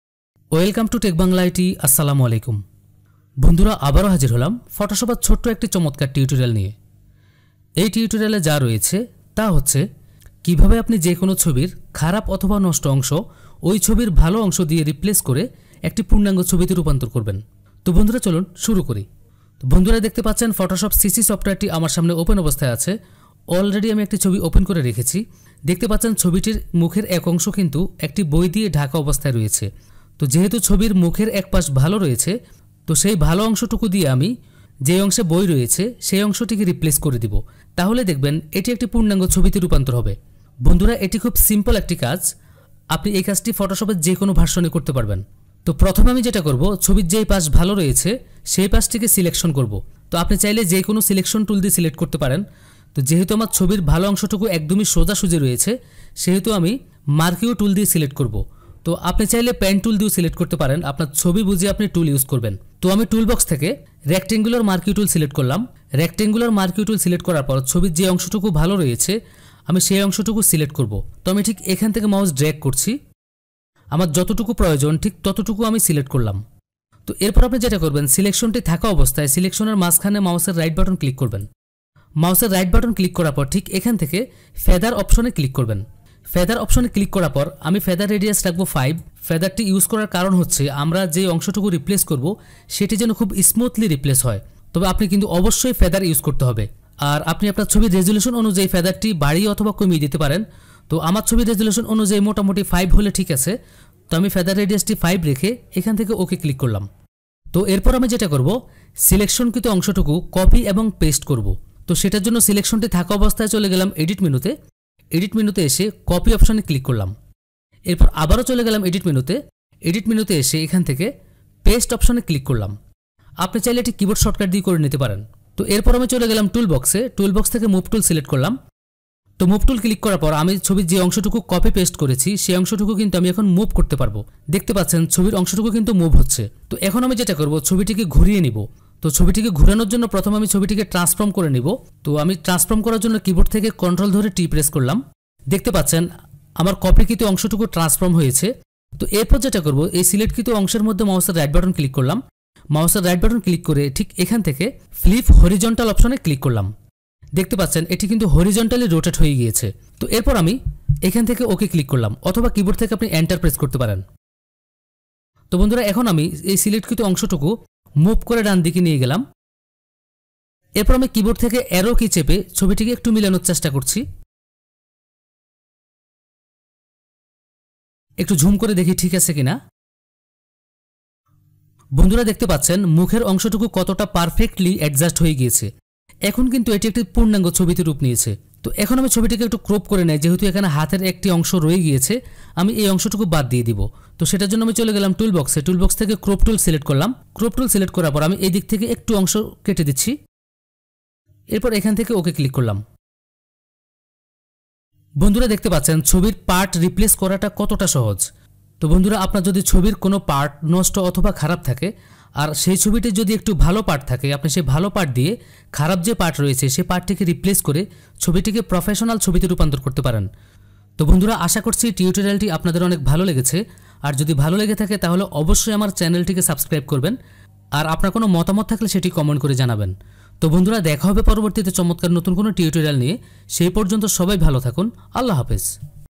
How to Replace Part of any Photo in Adobe Photoshop : fix any damaged Photo
Hi Friends
In this tutorial I have explained about How to replace  part of the any Photo in Adobe Photoshop CC in Bangla

In this Photoshop CC Tutorial you will see about How to fix any damaged Photo